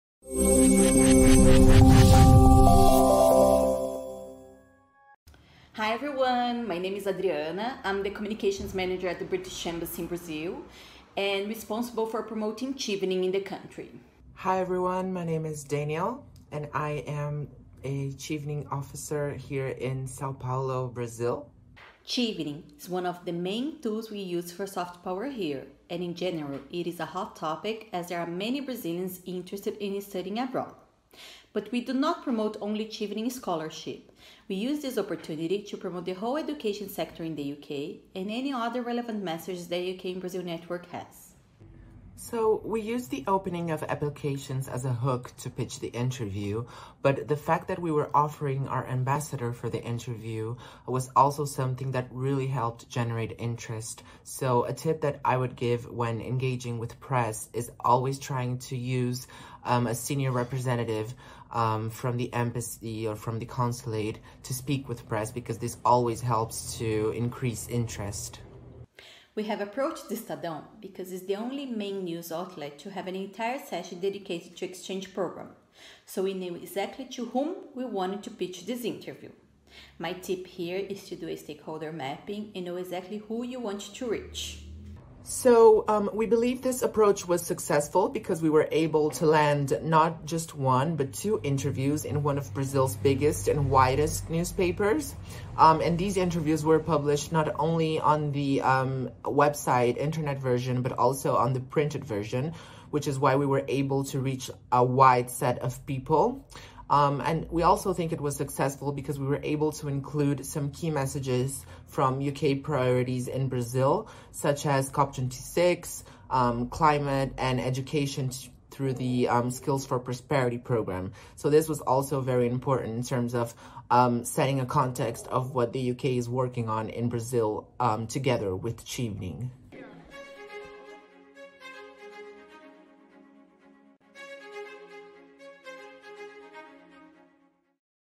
Chevening Conference Brazil
Looking for ideas to promote Chevening digitally? Communications Manager and Chevening Officer at the British Embassy in Brazil share how they approached one of the biggest news producers in Brazil to run a feature on the Chevening programme.